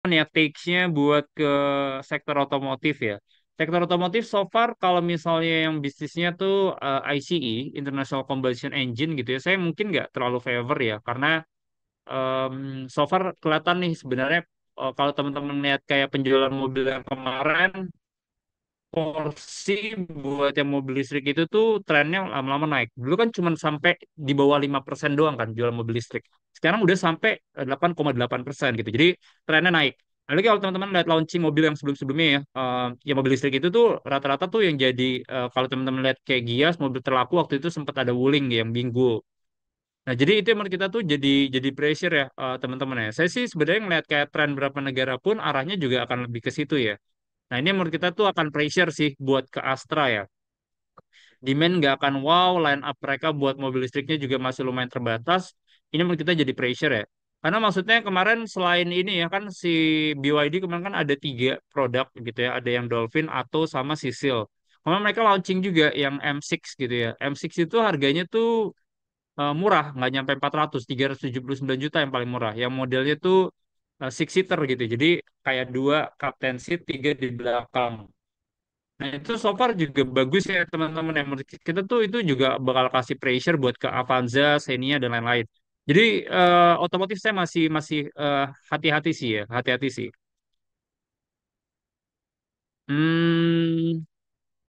Mobil Listrik dan Industri Otomotif
Video ini merupakan cuplikan dari webinar rutin Komunitas HungryStock yang diadakan setiap Sabtu pagi.

Pada acara tanggal 20 Juli 2024, HungryStock berkesempatan untuk bertemu dengan Pak Nico Laurens selaku Honorary Researcher HungryStock & Head of Research dari Panin Sekuritas pada acara StockScope.

Untuk bergabung bersama komunitas HungryStock bisa melihat info lebih lanjut di